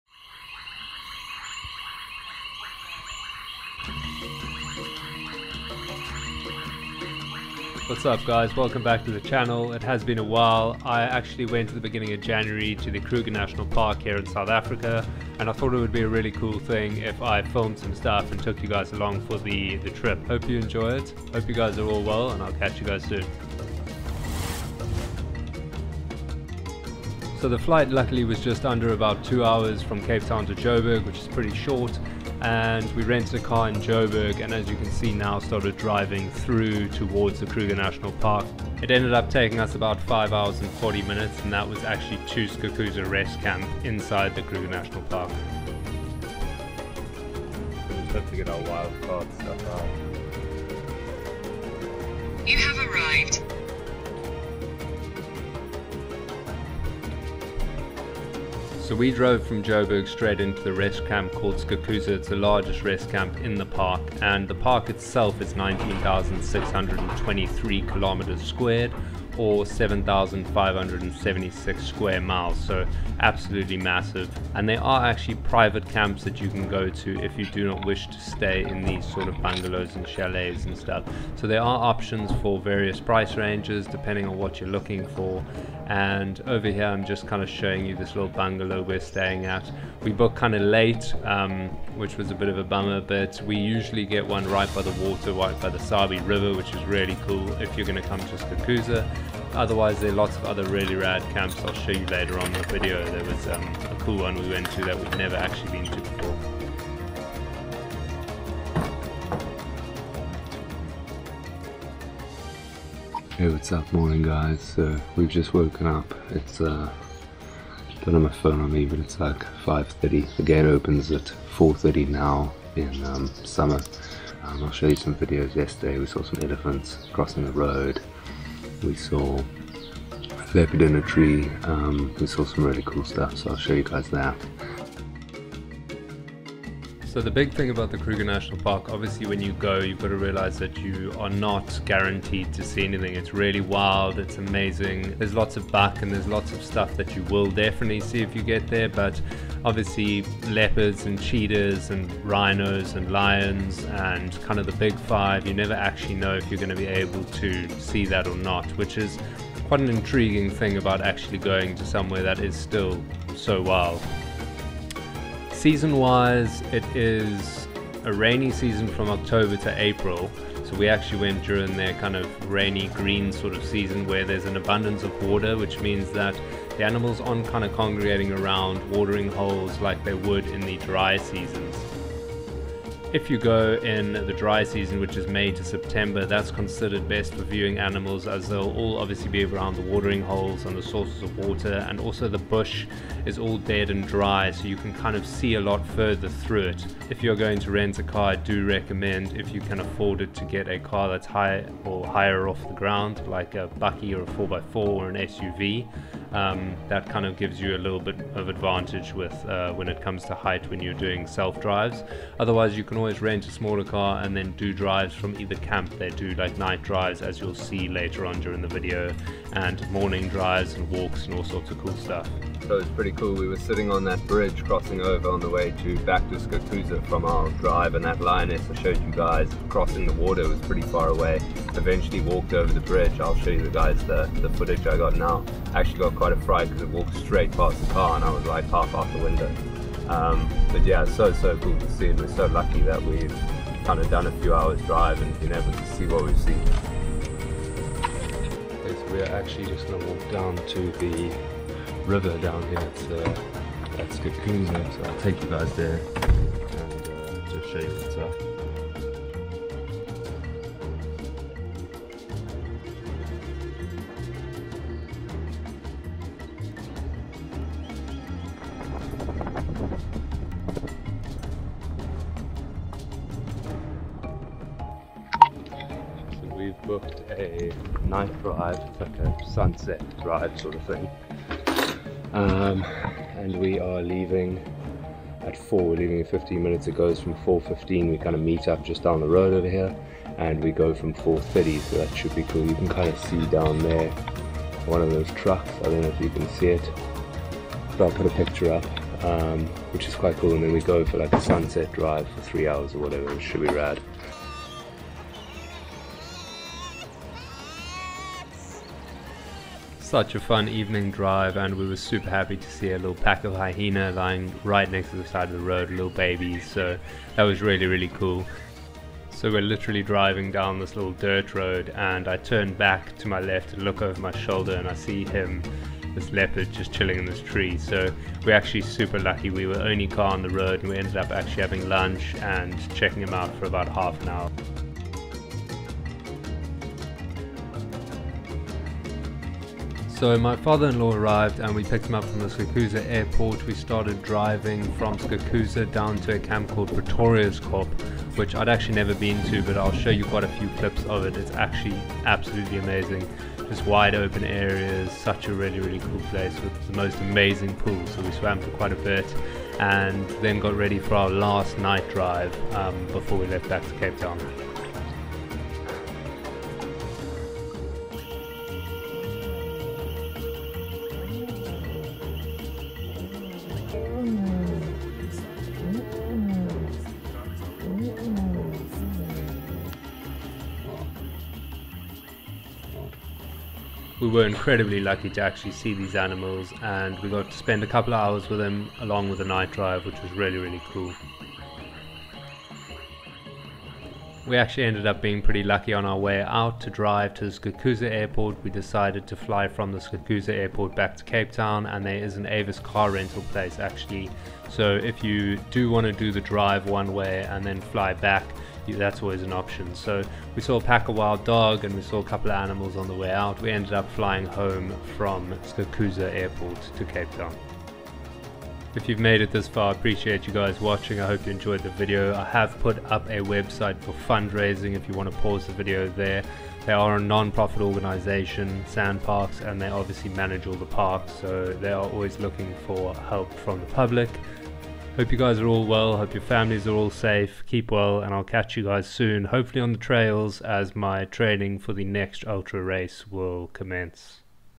Amazing Wildlife! Kruger Park Trip 2022
Incredible Trip to the Kruger National Park in South Africa!
I thought since most of my views come from other parts in the world I'd film our trip and share it with you guys.

If you are looking to support an organisation follow the